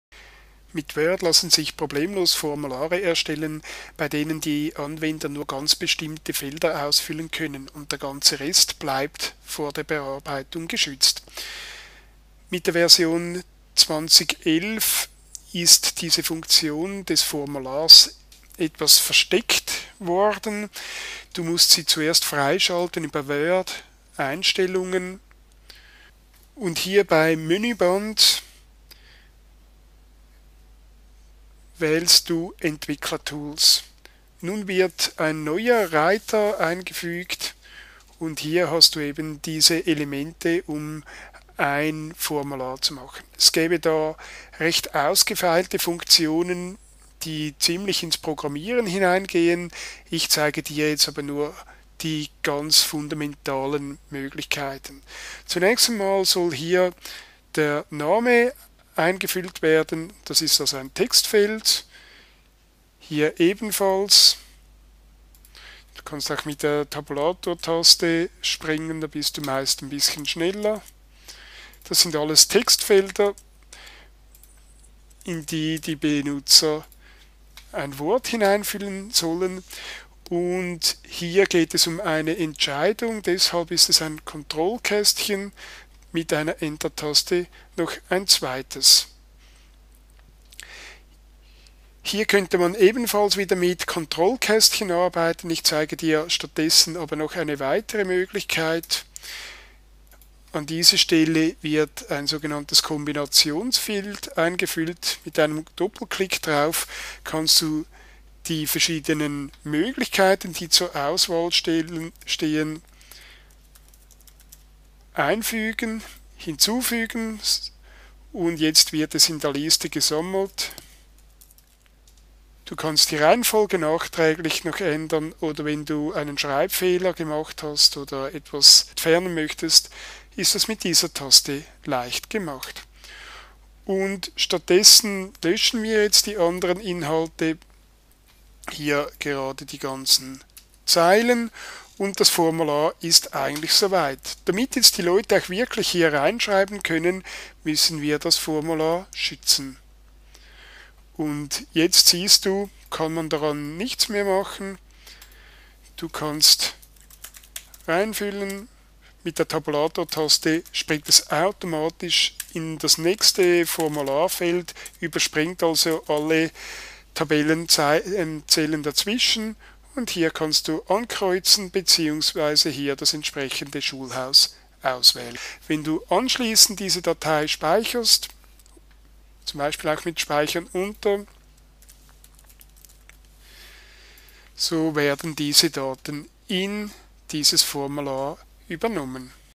Word-Formular
In einem Word-Formular können andere Personen nur in gewissen Feldern Eingaben machen, die restlichen Inhalte sind geschützt. Die Formularfunktion befindet sich in den Entwicklertools, die man erst im Menüband aktivieren muss.